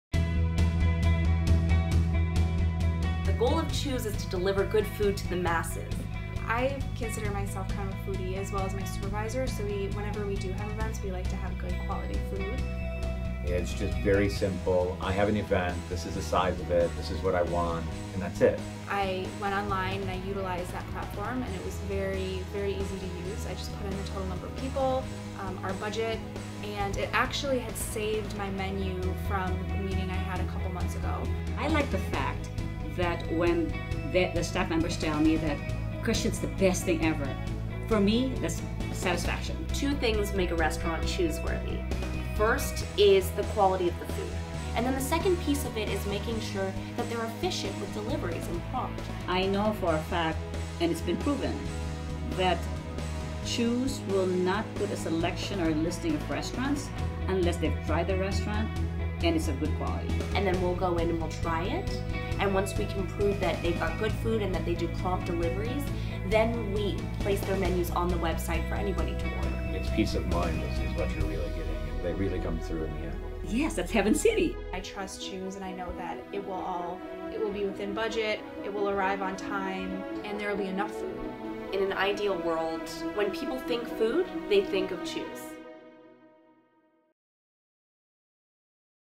Customer Testimonials
Real customers explain why they love Chewse.